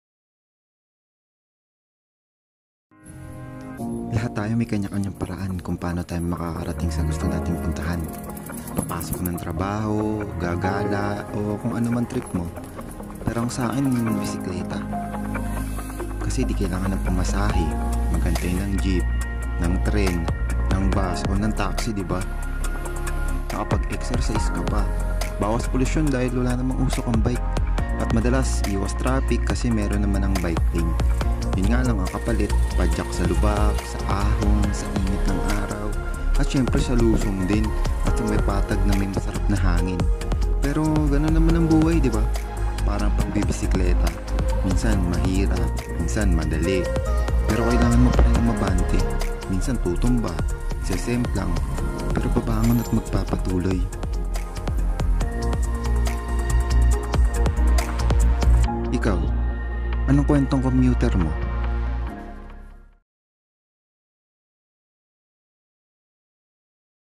Kwentong Komyuter by John Wendell Pepito | Freedom Vlog - Ang Kwentong Komyuter
The video is about the daily commuting story of a guy who uses his bike to get where he wants.
He will also feature the ups and downs of riding a bicycle and how it is similar and related to
how life goes.